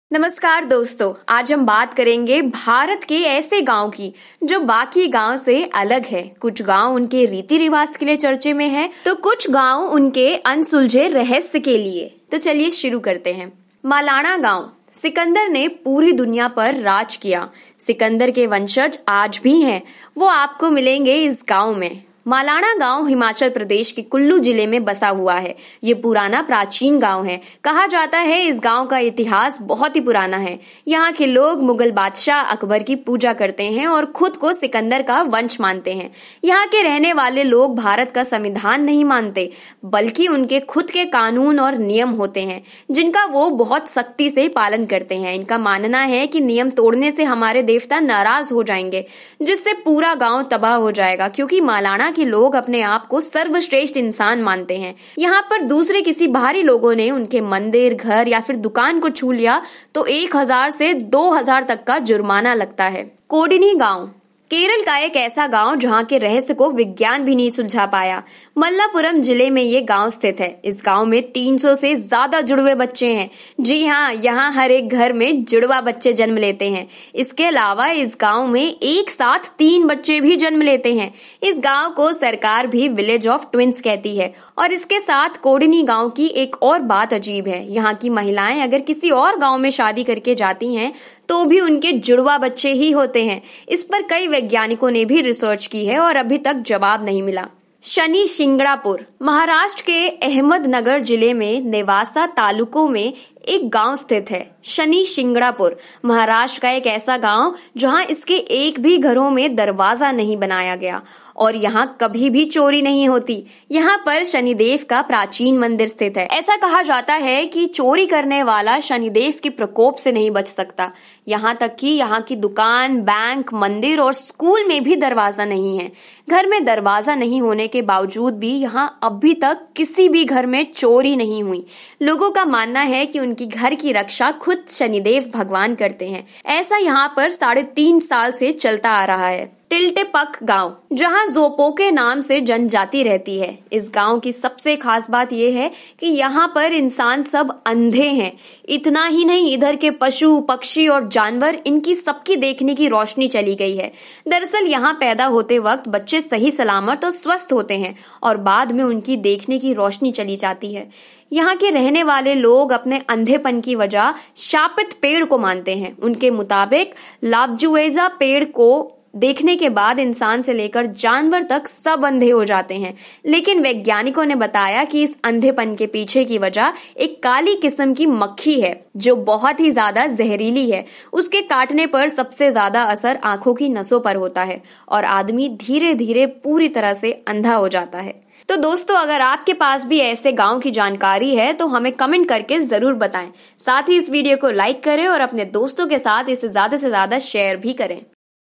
जानें दुनिया के सबसे रहस्यमय गाँव के बारे में I Knowledge Club
आज हम बात करेंगे दुनिया के ऐसे गाँव की जो बाकी गाँव से अलग है। कुछ गाँव उनके रीती रिवाज़ के लिए चर्चे में है तो कुछ गाँव उनके अनसुलझे रहस्य के लिए